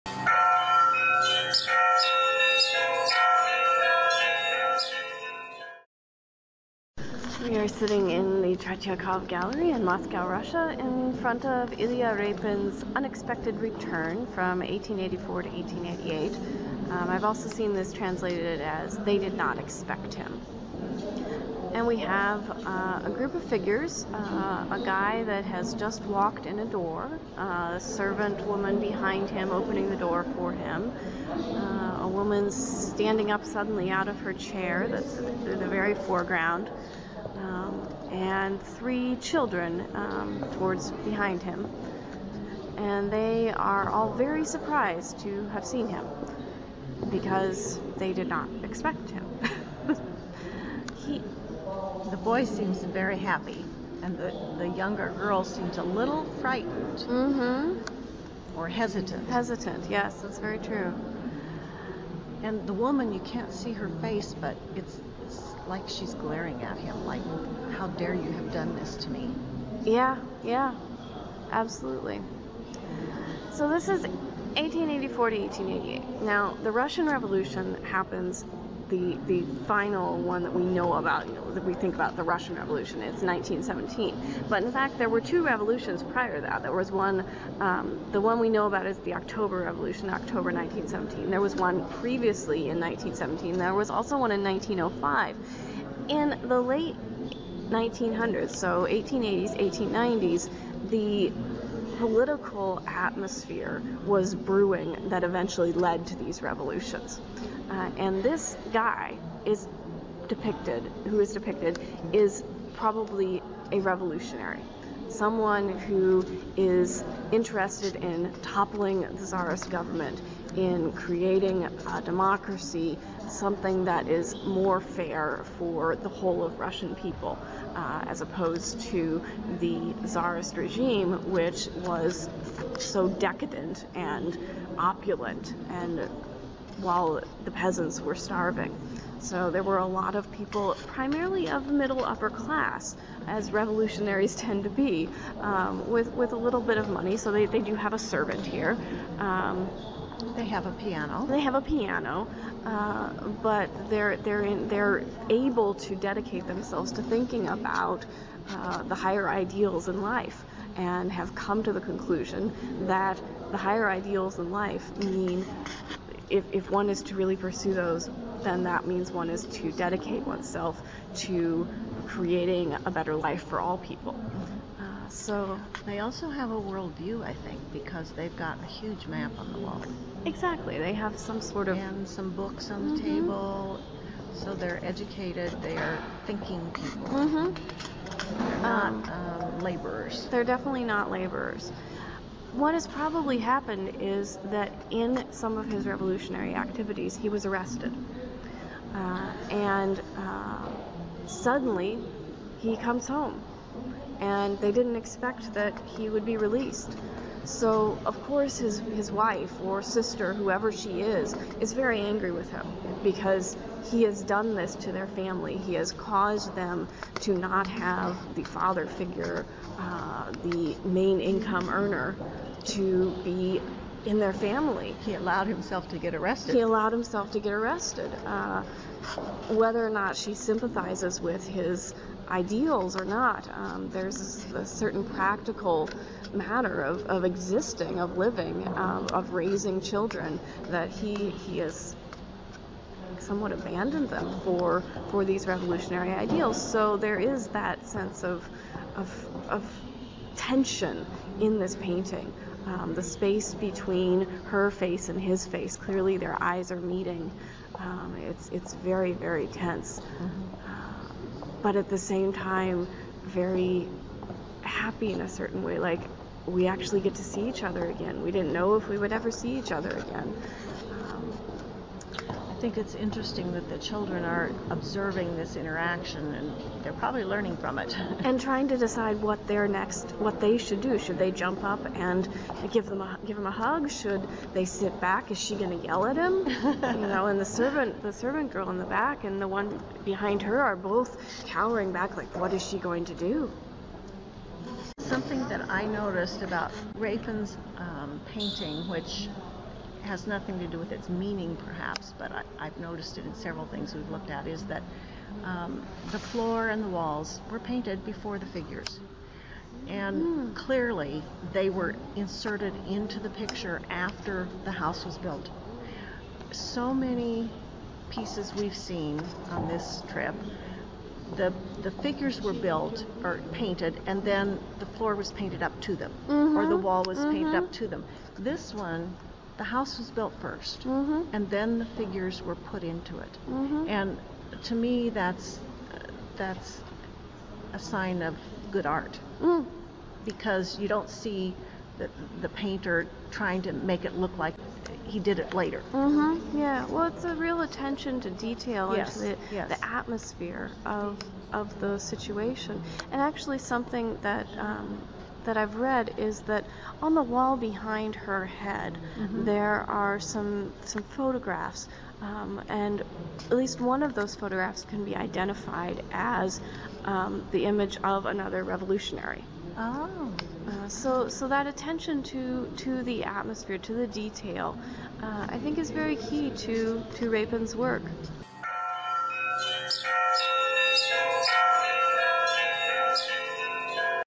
Repin, Unexpected Return, 1884-1888
Ilya Repin, Unexpected Return, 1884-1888, oil on canvas, 160 x 167 cm (State Tretiakov Gallery, Moscow)

Speakers:  Dr. Marie Gasper-Hulvat and Dr. Juli-Ann Gasper

Video production supported by a grant from the University Teaching Council at Kent State University.